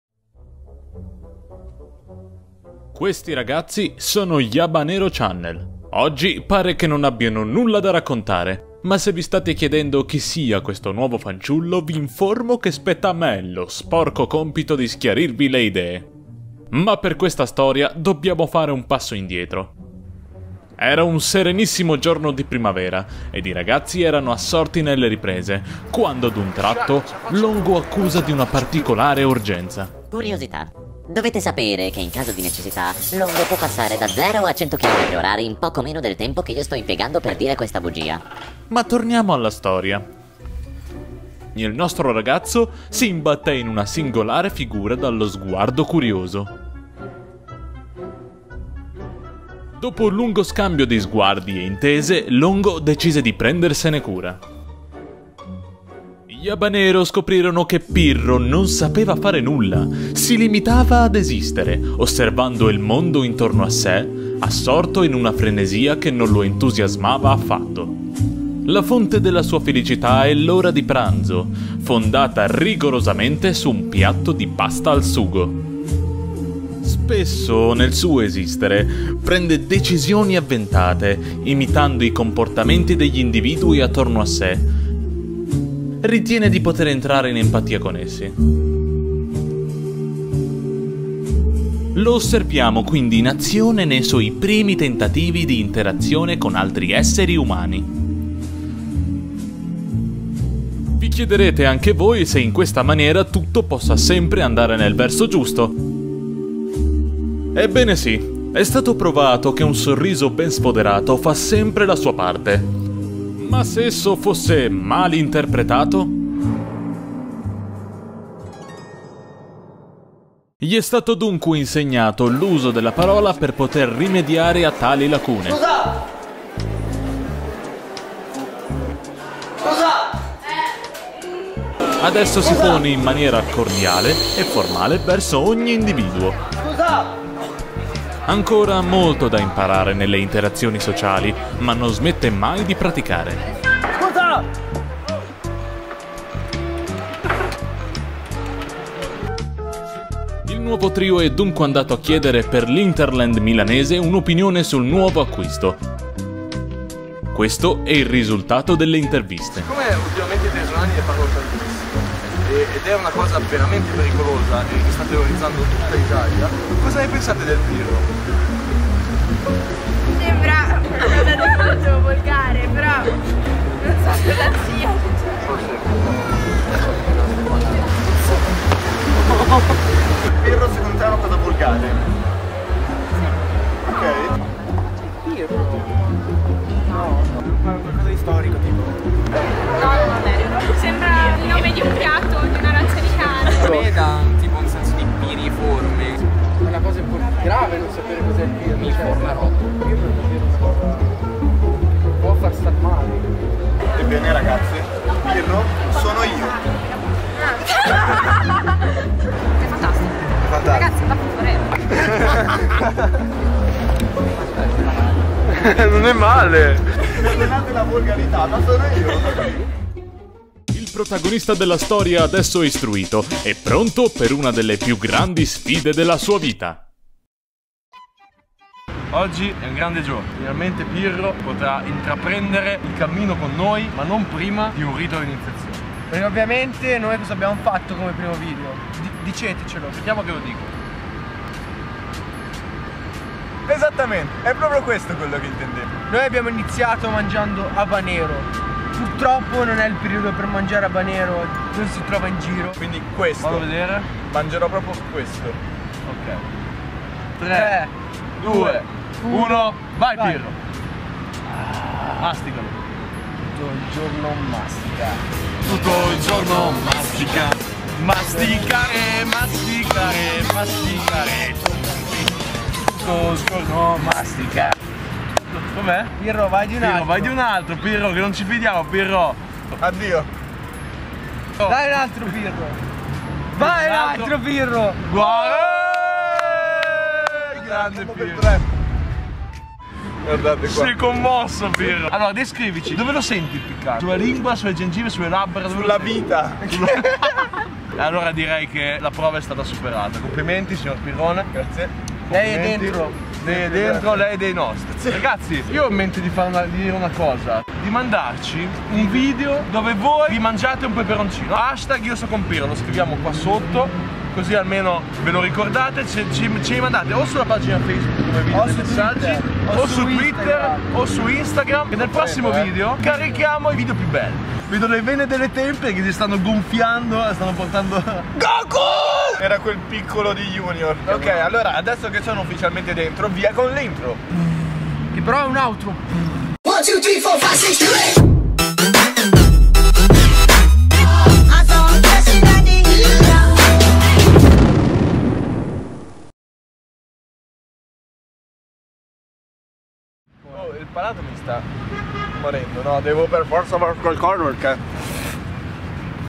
Un PIRATA su Habanero Channel
Non tutte le storie hanno un lieto fine, quella che stiamo per raccontarvi ha dell'incredibile! Ringraziamo Ludo Thorn per averci prestato la voce, passate sul suo canale: